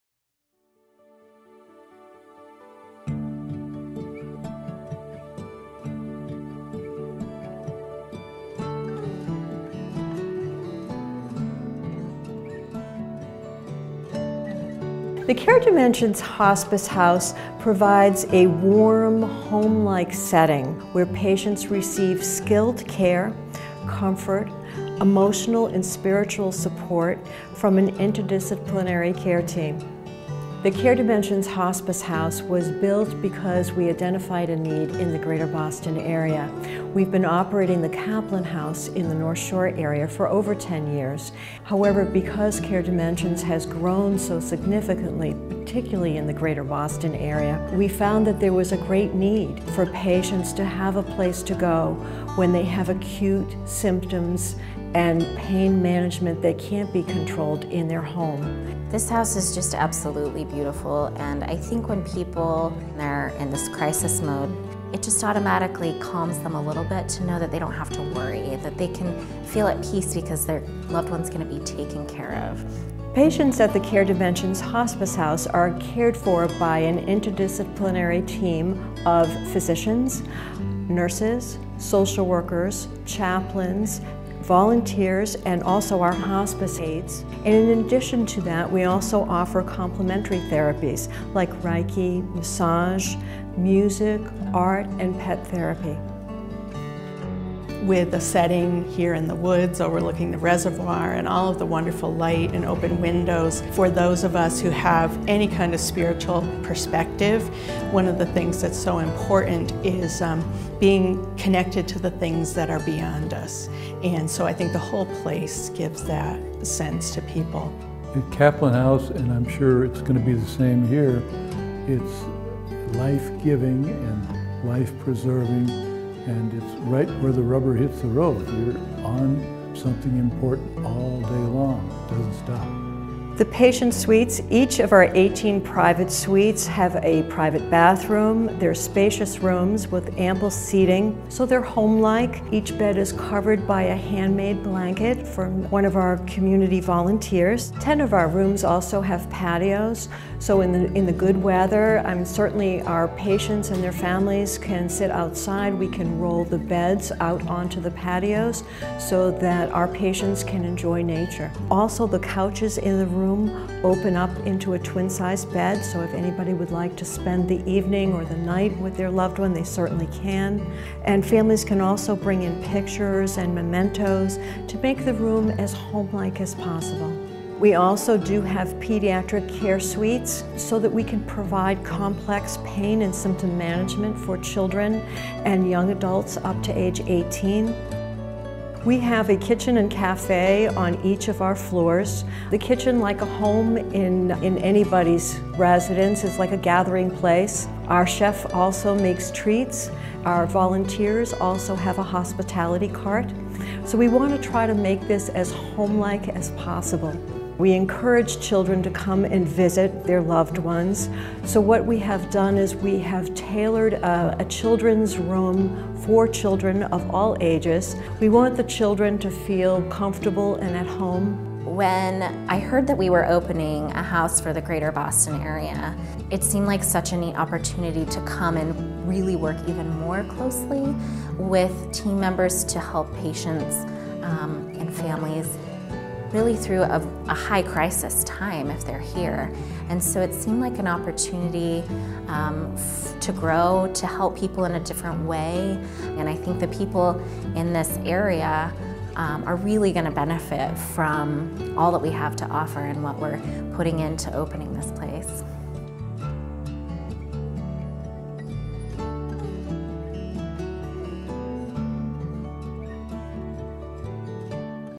Care Dimensions Hospice House Video Tour
A video tour of the Care Dimensions Hospice House in Lincoln, MA, an 18-bed inpatient hospice facility operated by Care Dimensions.